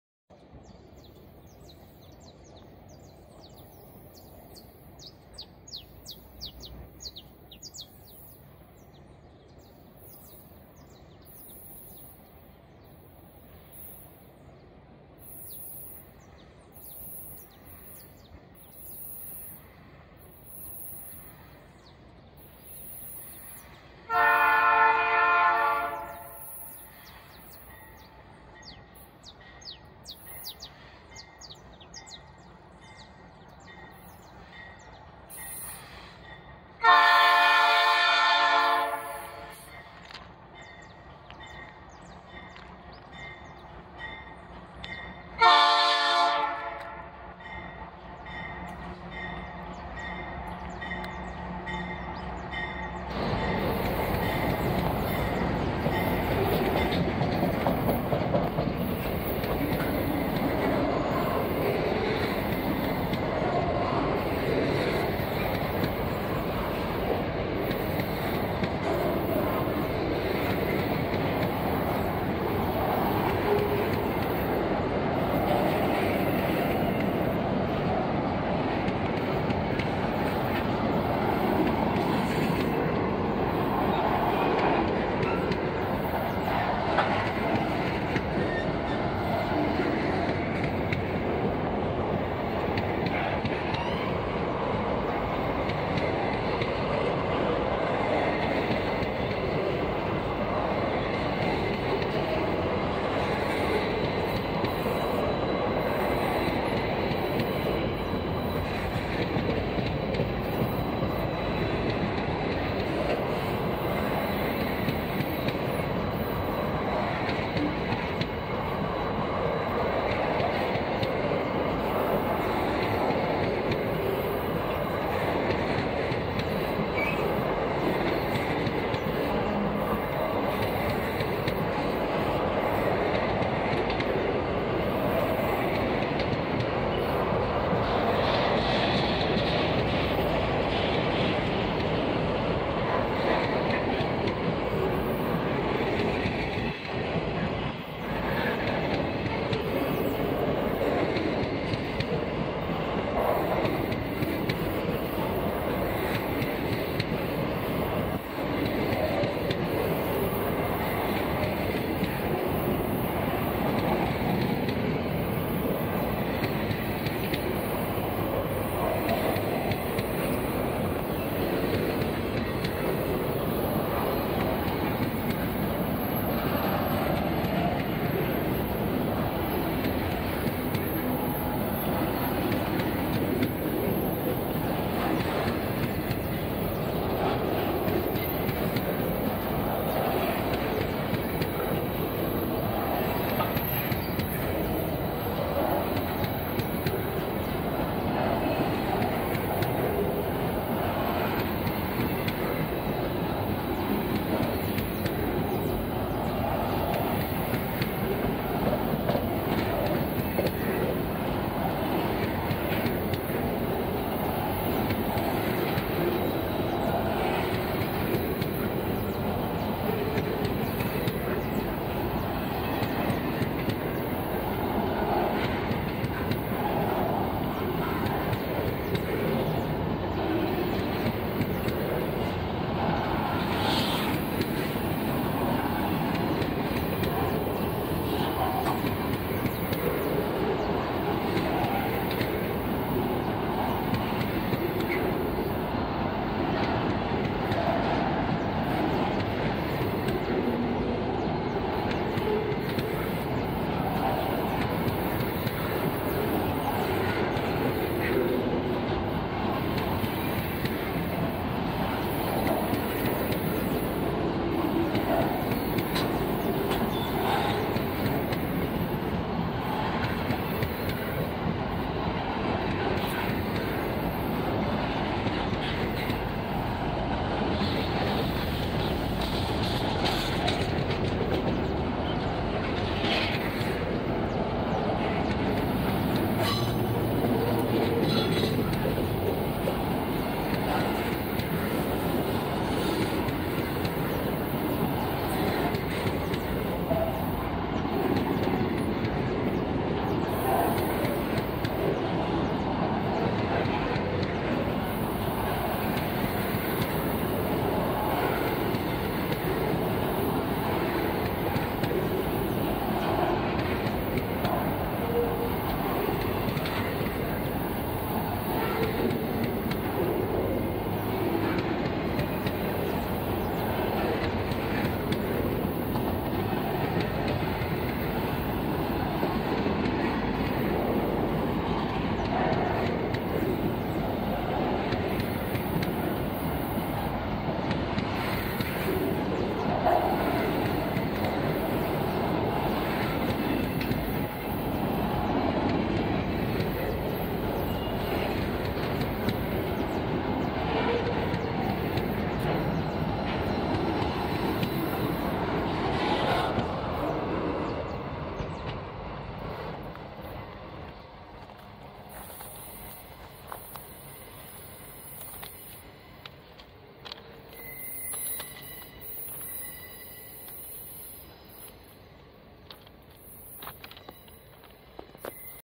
Composição entrando na curva com cautela
Um trecho com várias passagens  de nível, os trens transitam com cautela